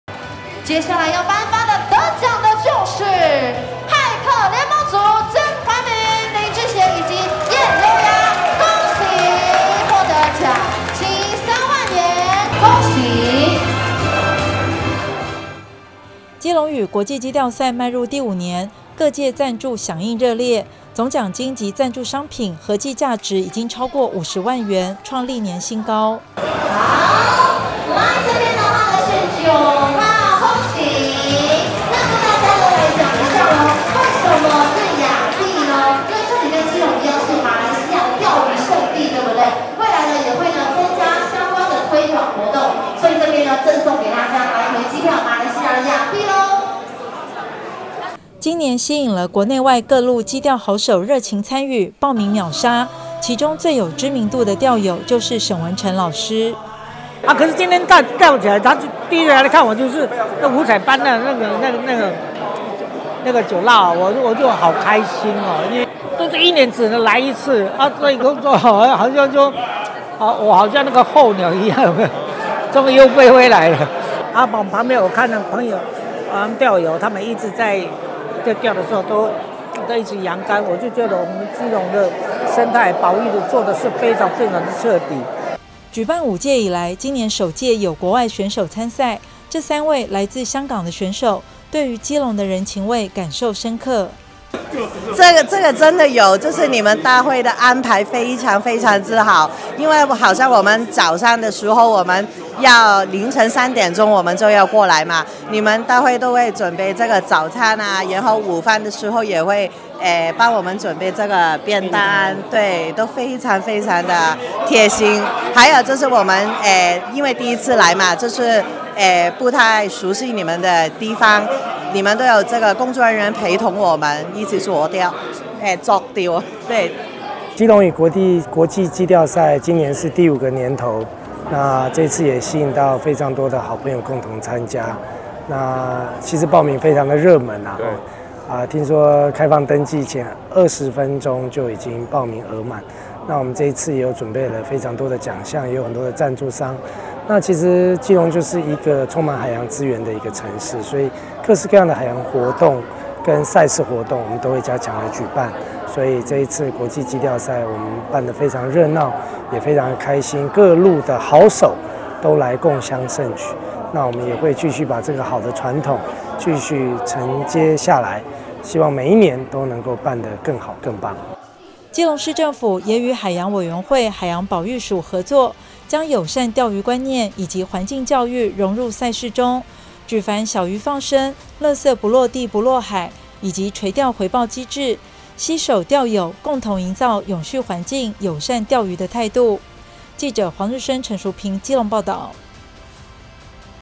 沈文程|基隆漁好大|參與基隆嶼磯釣賽 知名藝人沈文程按讚| 基隆嶼磯釣賽選手之夜 團體賽冠軍出爐
備受關注的「2023基隆嶼國際磯釣賽」隆重登場，謝國樑市長特別到選手之夜現場，頒發獎項並且感謝國內外磯釣好手共襄盛舉，來推廣臺灣釣魚運動，讓世界看見「臺灣龍珠」基隆嶼的美麗。

基隆嶼國際磯釣賽邁入第五年，各界贊助響應熱烈，總獎金及贊助商品合計價值已超過50萬元，創歷年新高

今年吸引了國內外各路磯釣好手熱情參與，報名秒殺，其中最有知名度的釣友就是沈文程老師。舉辦五屆以來，今年首屆有國外選手參賽，這3位來自香港的選手，對於基隆的人情味感受深刻。

基隆市政府也與海洋委員會海洋保育署合作，將友善釣魚觀念及環境教育融入賽事中，舉凡小魚放生、垃圾不落地不落海以及垂釣回報機制，攜手釣友共同營造「永續環境‧友善釣魚」態度。